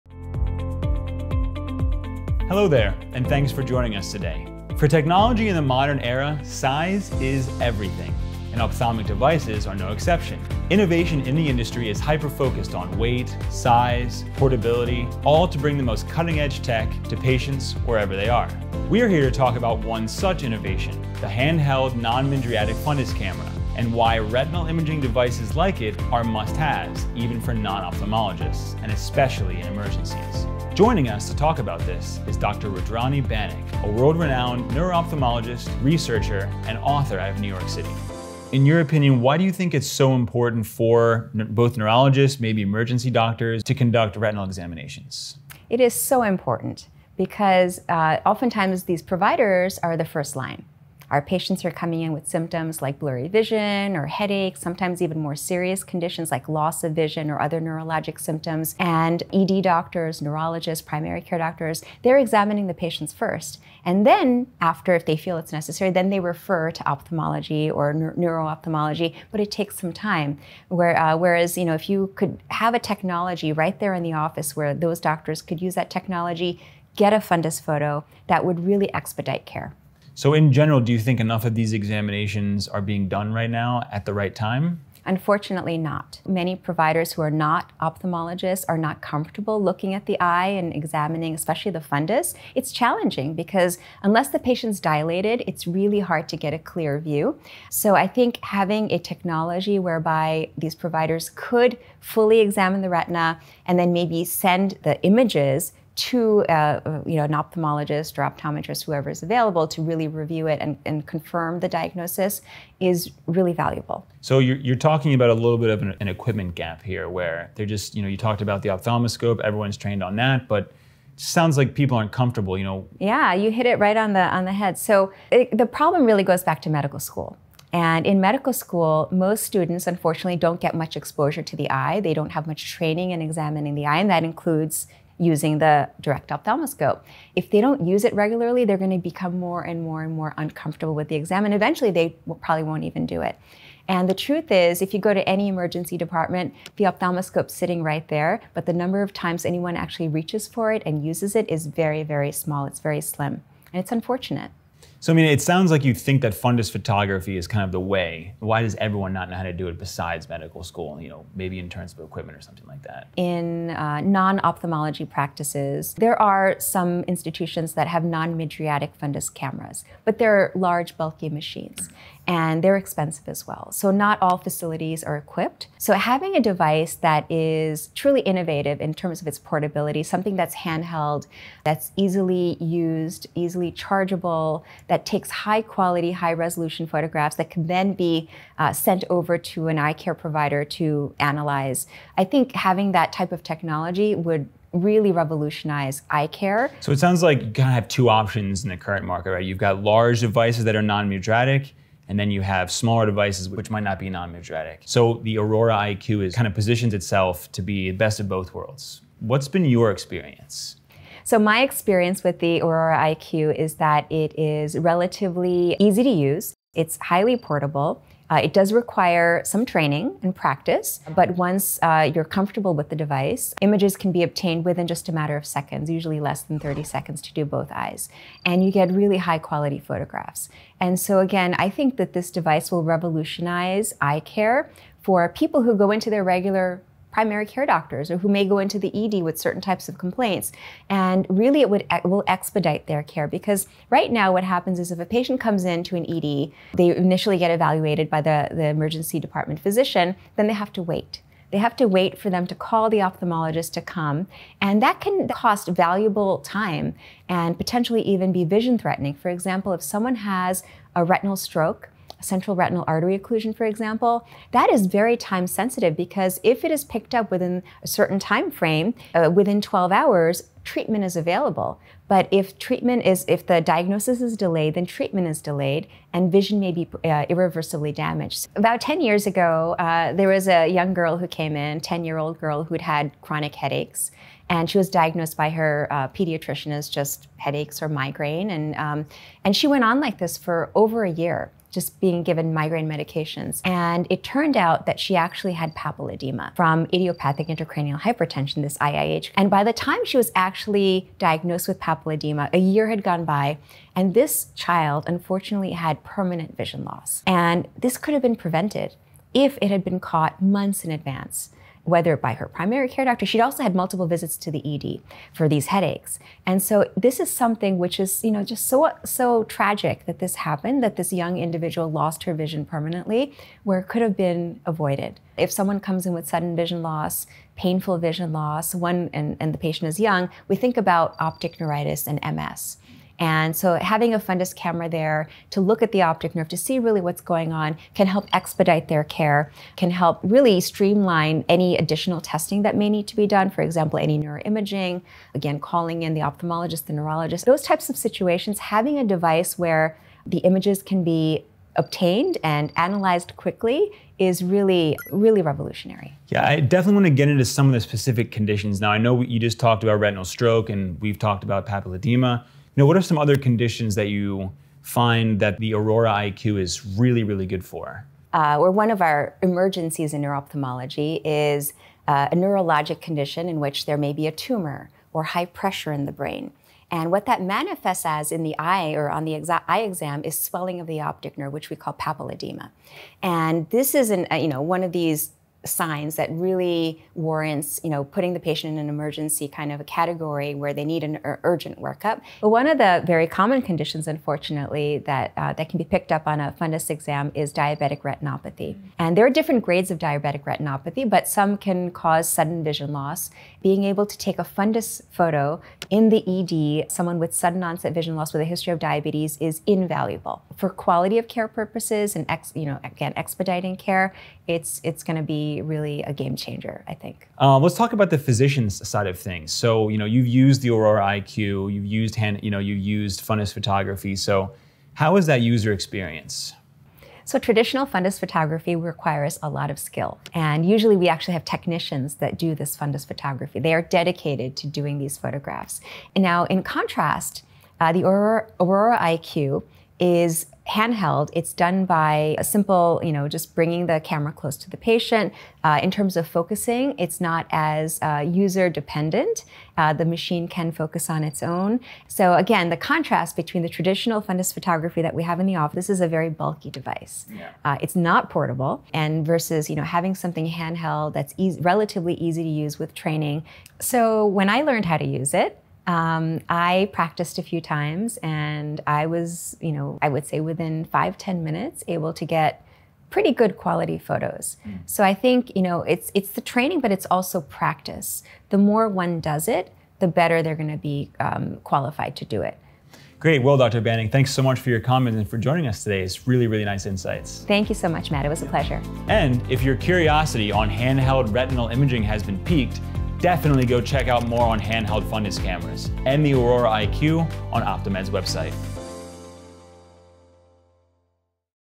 Exploring the potential of Handheld Fundus Cameras in Neurology and ED with Dr. Rudrani Banik, M.D.
Exploring the potential of Handheld Fundus Cameras in Neurology and ED – Interview with Dr. Rudrani Banik, M.D.

In a recent interview Dr. Rudrani Banik describes the importance of retinal examination in Neurology and ED and how handheld fundus cameras like the non-mydriatic Optomed Aurora® IQ can improve the detection of conditions like papilledema in these environments.

The future of fundus photography is flexibility and portability – for more, head on over to our